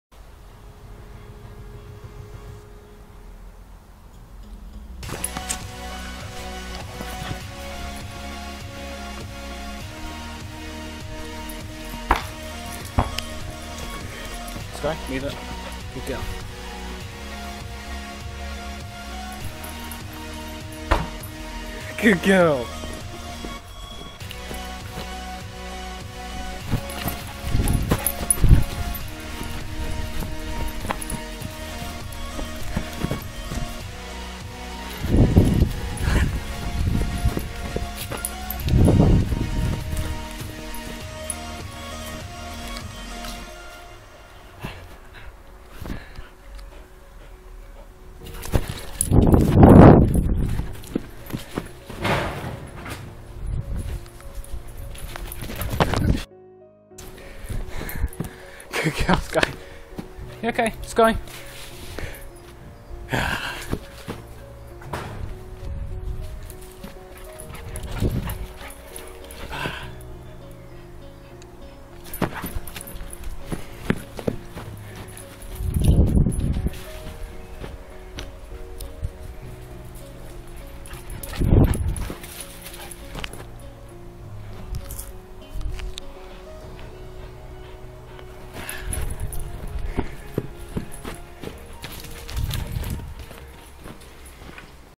Skyes Playtime
A video of myself and Skye playing in the garden.
Recorded on phone - explaining quality